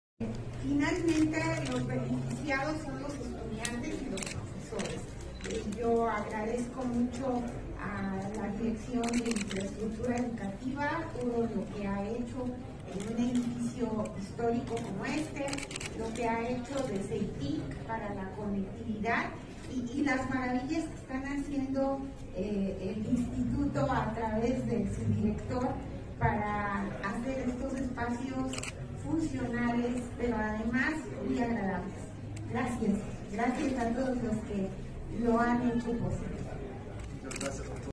Lilia Cedillo destacó los beneficios que esta restauración traerá para la comunidad universitaria
Después de un recorrido por la recién reinaugurada Casa Amarilla de la BUAP, la rectora María Lilia Cedillo Ramírez destacó los beneficios que esta restauración traerá para la comunidad universitaria.